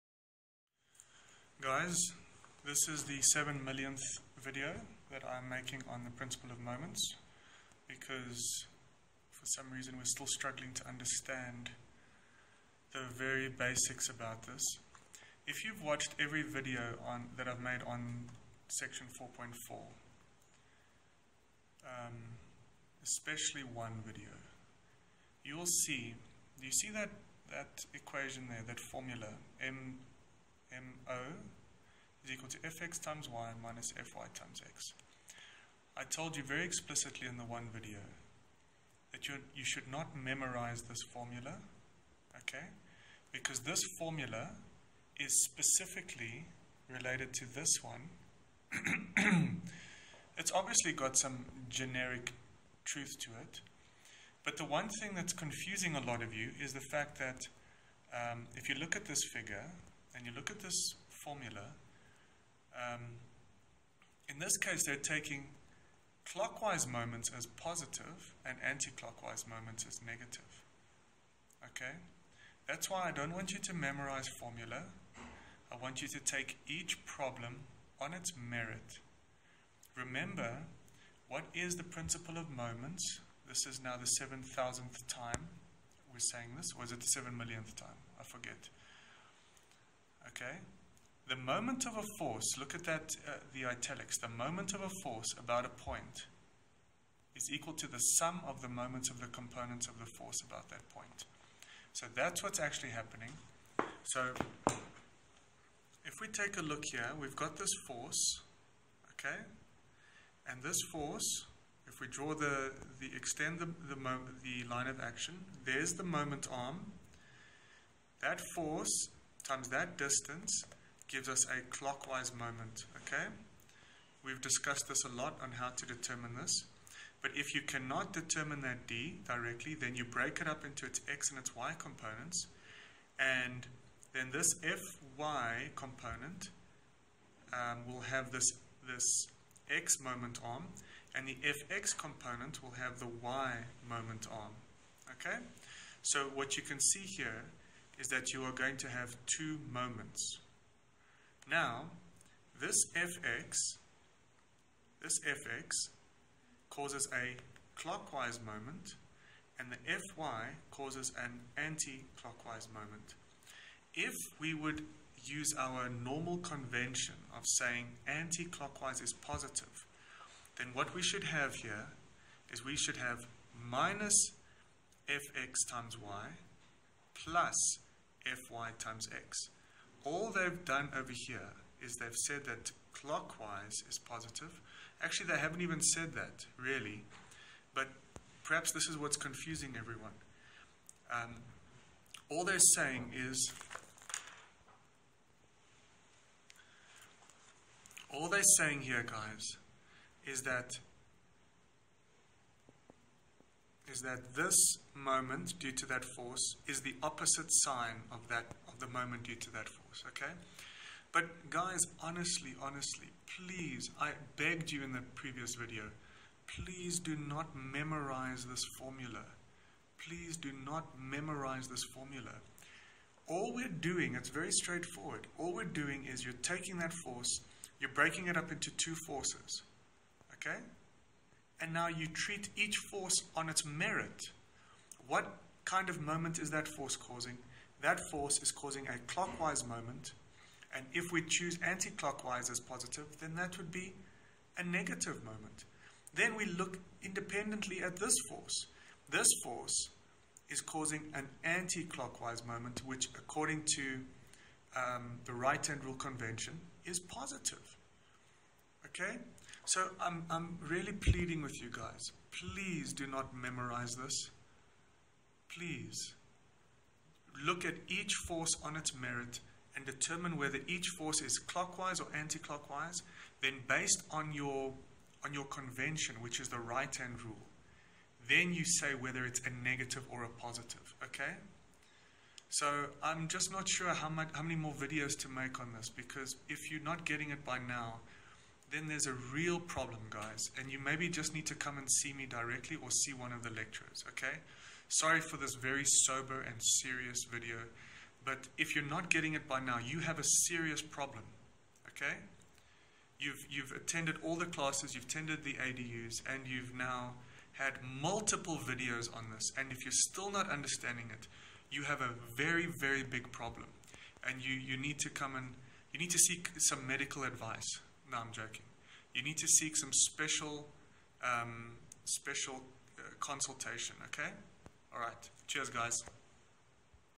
Chap 4.4 Discussion on formula: Mo = FxY- FyX (pg 132)
Brief discussion on the formula on pg 132:  Mo= FxY - FyX.